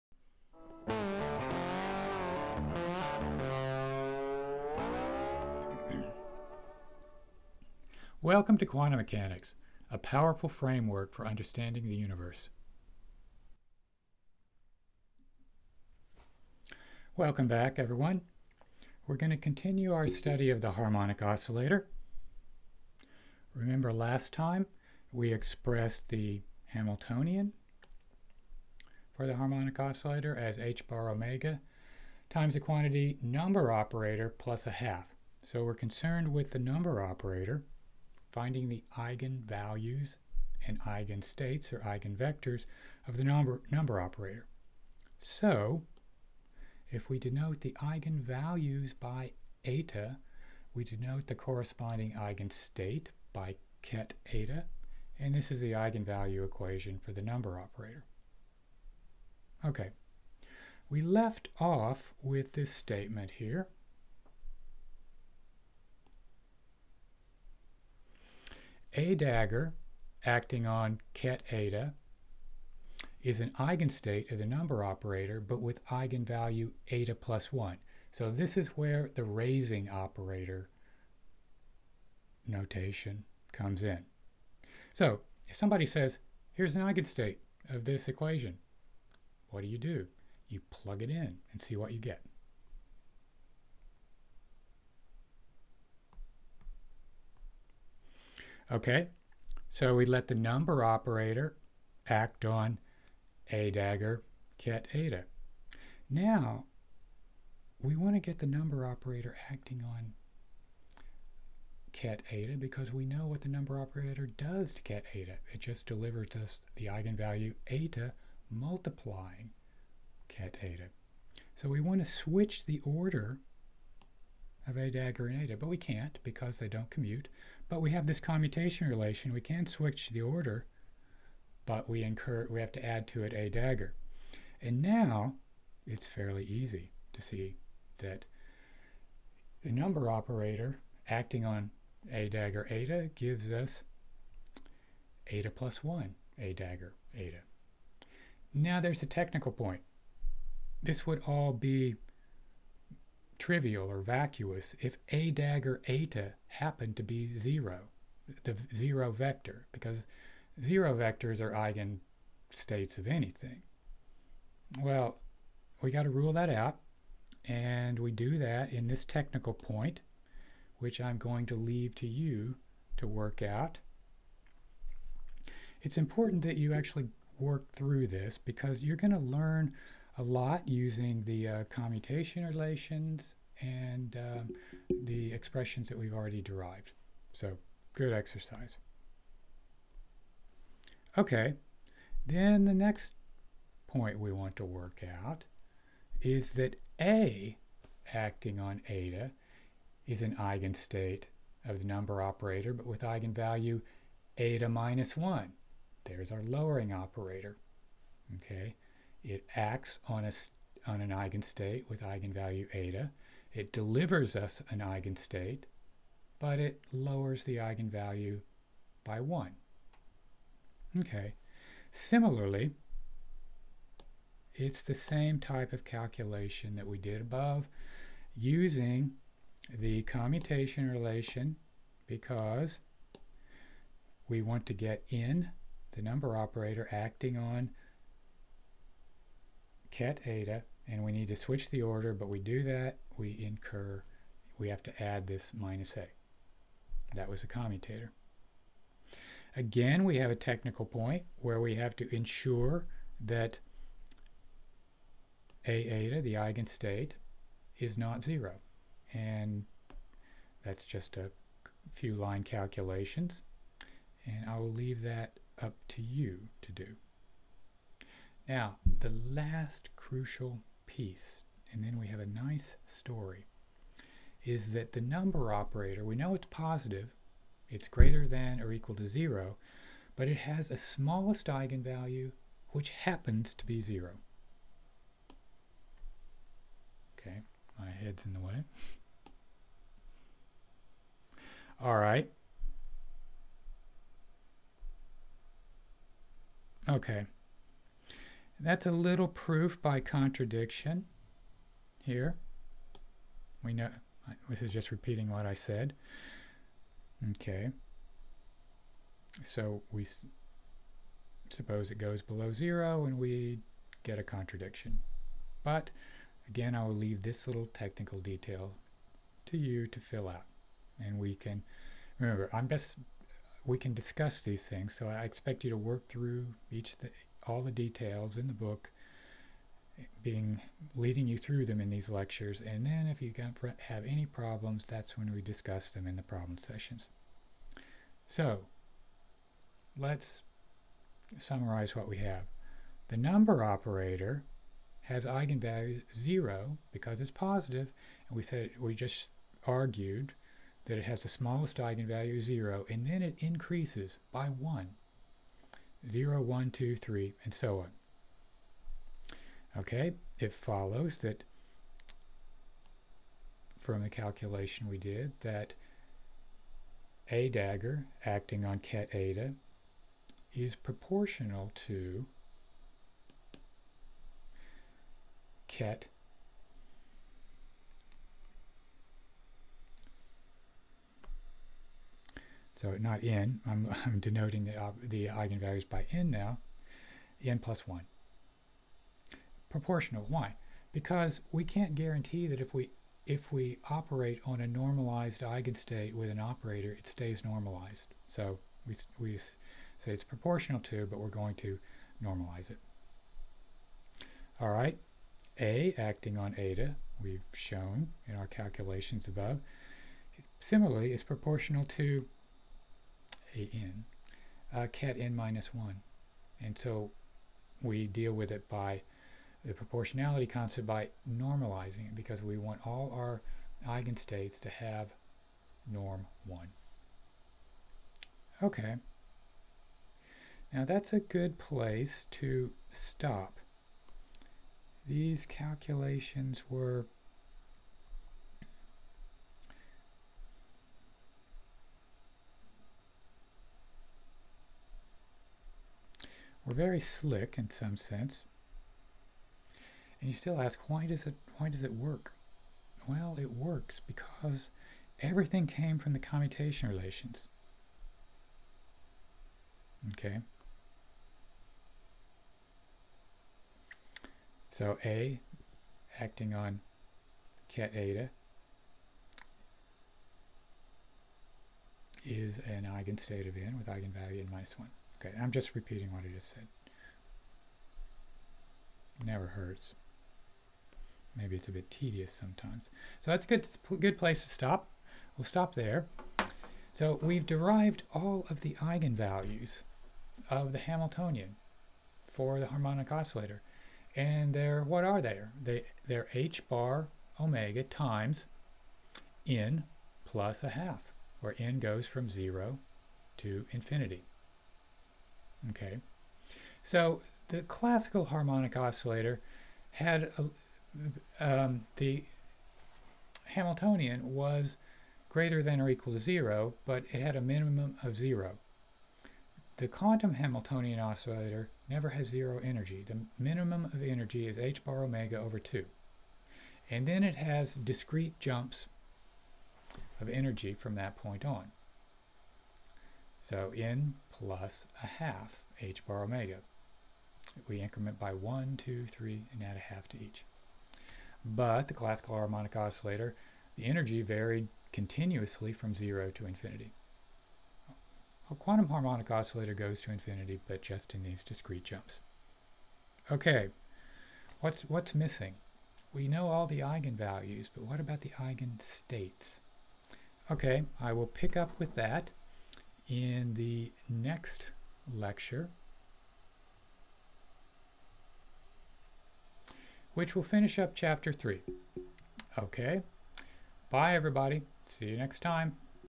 Quantum Mechanics Ch. 3: The Harmonic oscillator, Part 2.
This is the eighth lecture for Chapter 3, where we continue our discussion of the harmonic oscillator using the method of raising and lowering operators.

The course follows the first four chapters of my open book:
— Wiggins, Stephen (2020): Elementary Quantum Mechanics. figshare.

Detailed solutions to all of the problems given in the book can be found here:
— Wiggins, Stephen (2020): Solutions to the Exercises in Elementary Quantum Mechanics. figshare.